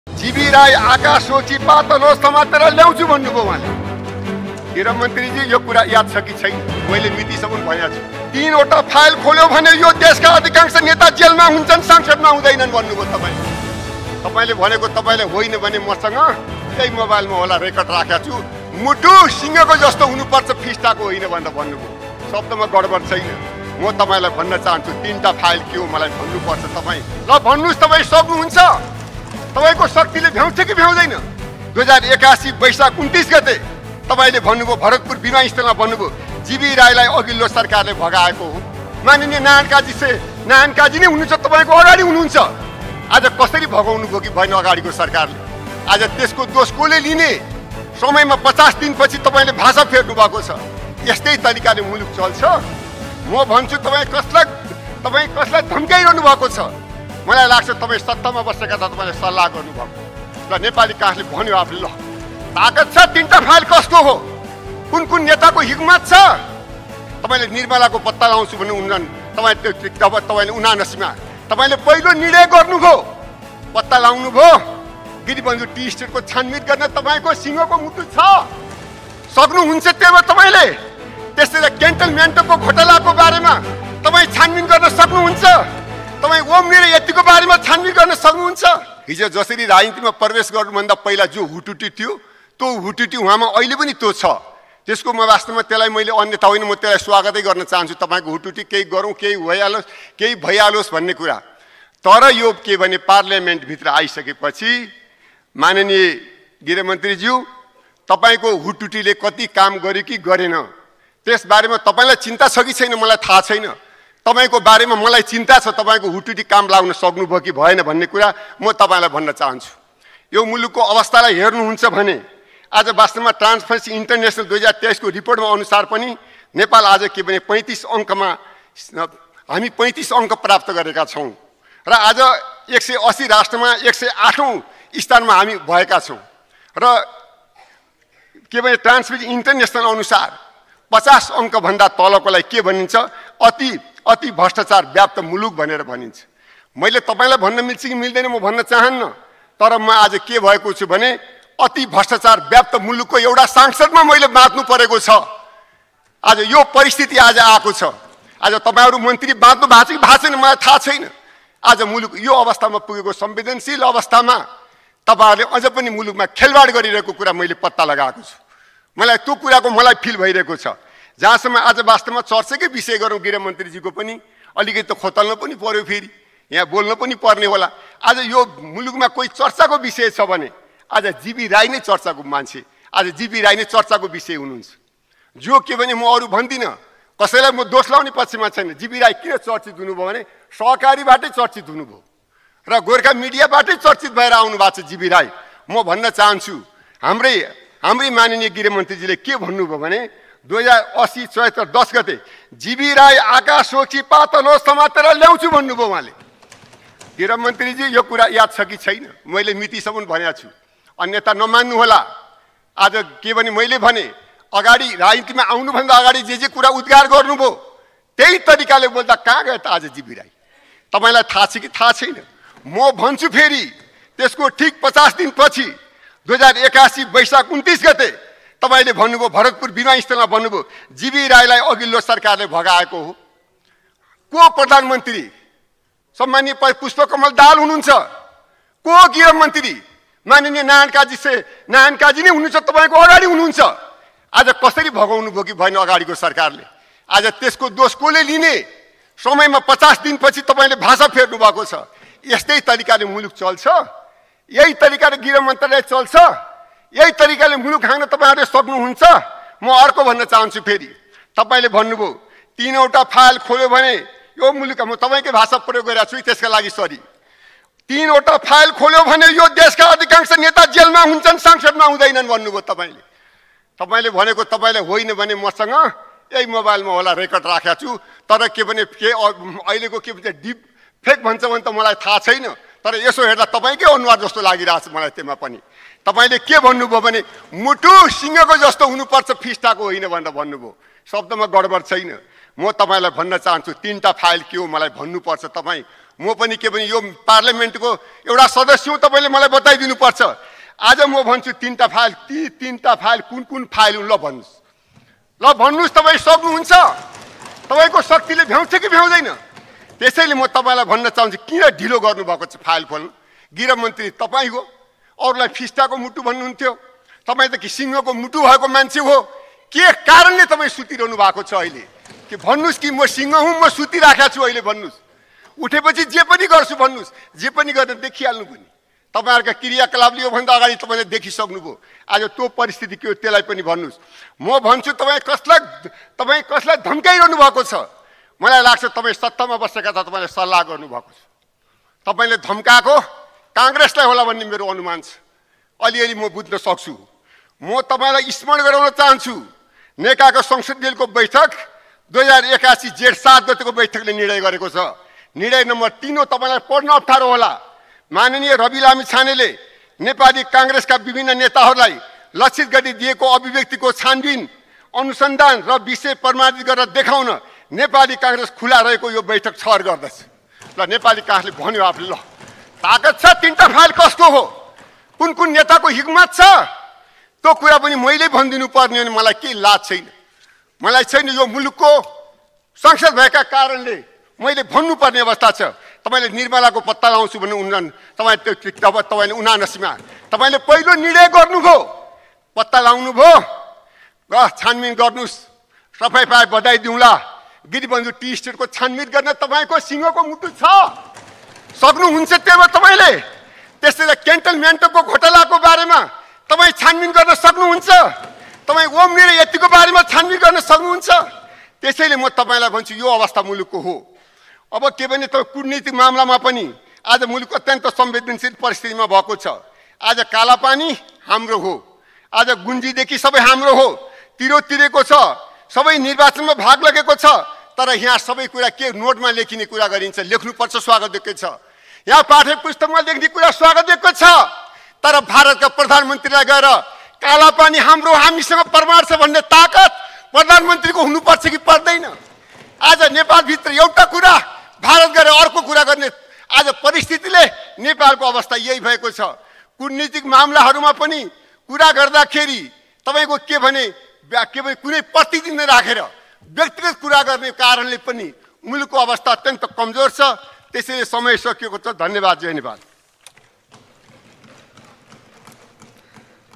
" 'जीबी राई आकाशमा होस् कि पातालमा, समातेर ल्याउँछु' भनेको याद छ कि छैन ? "
गृहमन्त्री रबी लामिछानेलाई हृदयराम थानीको प्रश्न;